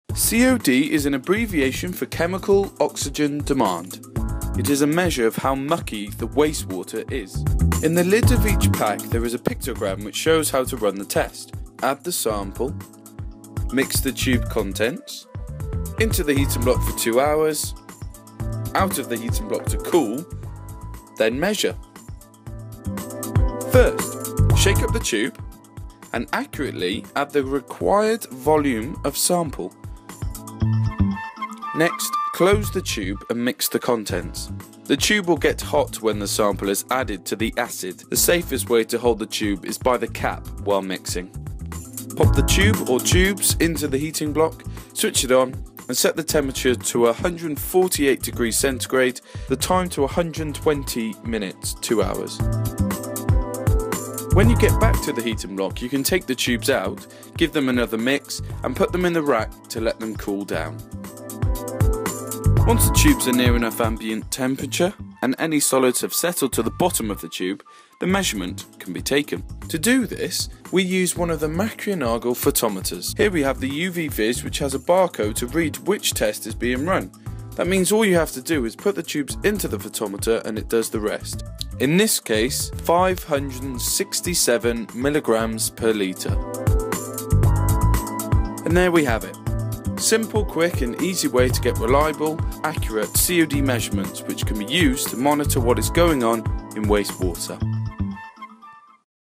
Chemical Oxygen Demand
Quick easy Chemical Oxygen Demand measurements can be made by anyone.